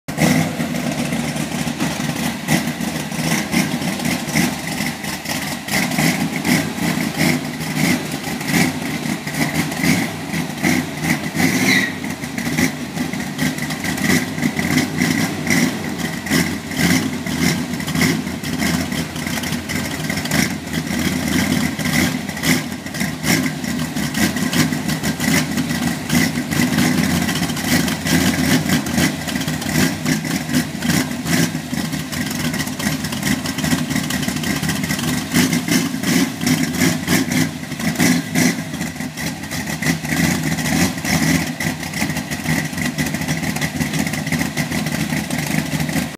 Backing out for a monthly warm up... All orginal 1970 Corvette 454 4-Speed
Billy filming me backing her out to let her run for a bit...